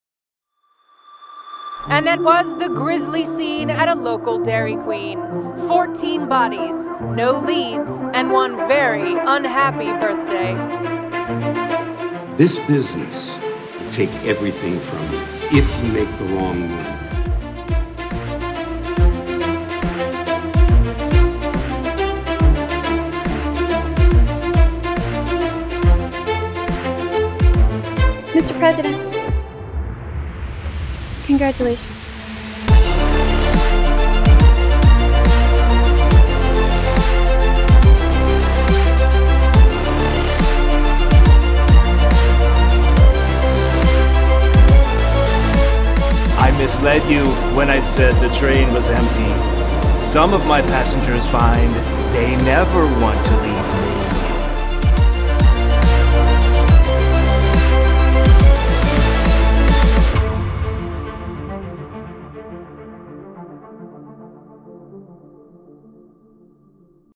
Aaron Landgraf - Production Reel
A brief collection of projects in which I have been fortunate enough to participate. Always available for contract work.